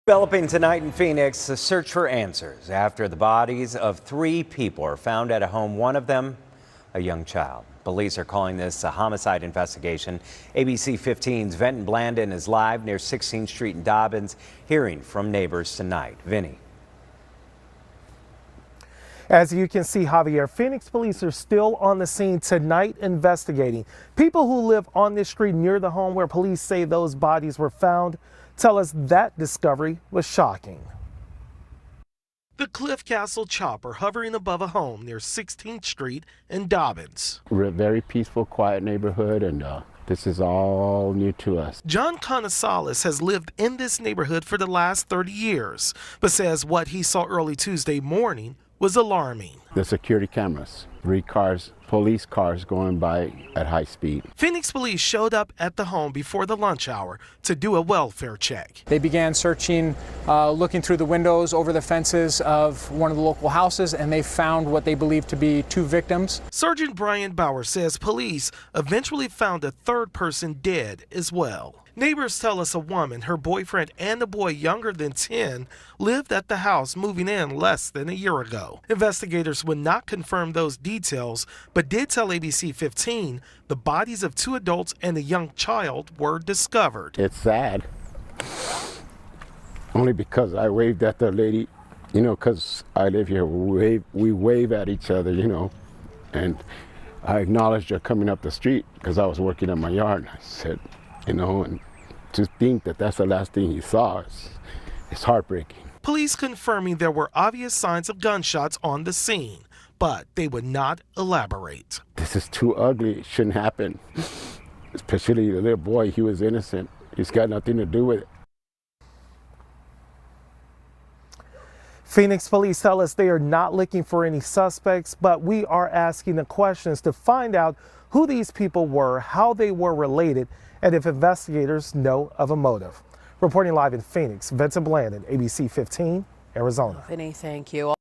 Three bodies found, including one of a young child in south Phoenix home
Phoenix police say officers responded to the home just before 11 a.m. after a welfare check was called in. When officers arrived, two adults and one child were found dead at the home. Officials say there are no suspects outstanding in this incident. Details surrounding the investigation have not yet been released, but police did say that there were obvious signs of gunshots at the scene.